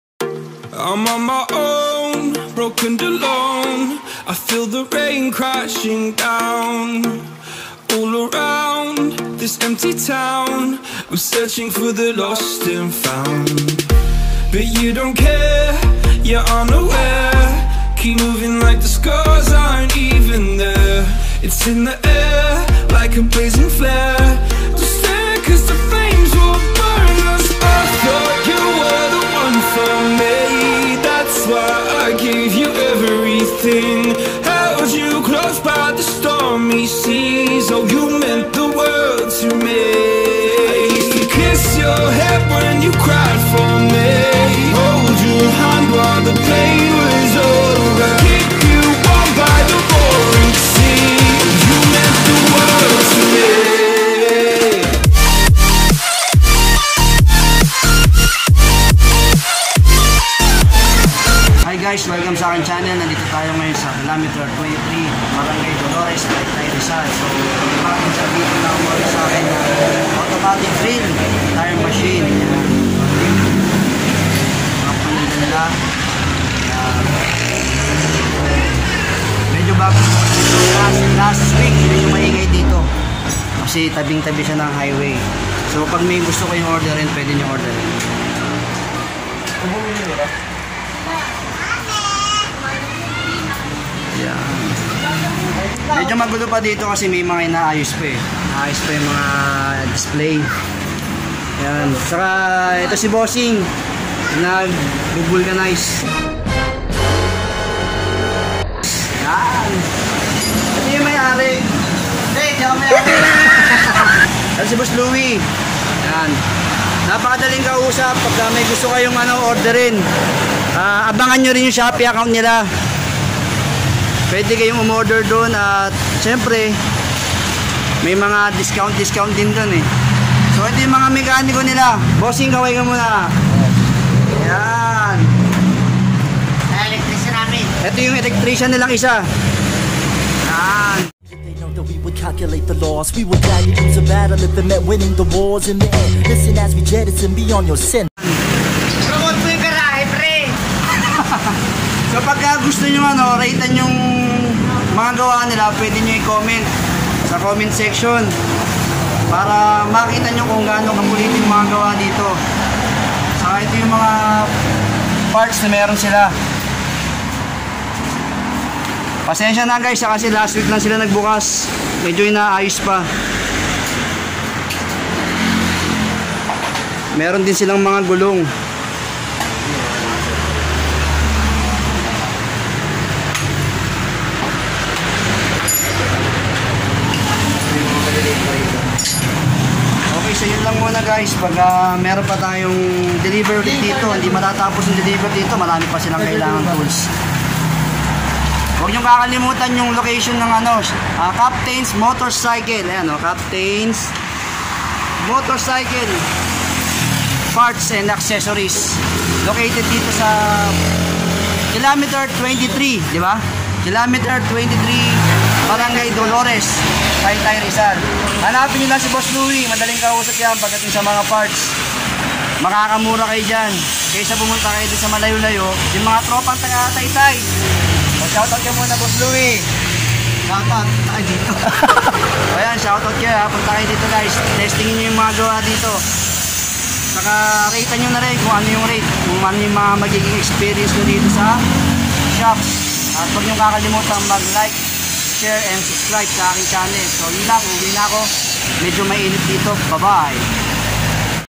DELIVER SA TAYTAY SA BAGONG MOTORSHOP
Newly opened Motorcycle Shop in Taytay
Visit na kayo sakanila mga boss.

Salamat po Captain's Motorshop sa tiwala sa RA Hardware Tools and Powertools

Like our videos.

Order na rin kayo samin ng tools and powertools!!

Till our next roadtrip and deliver!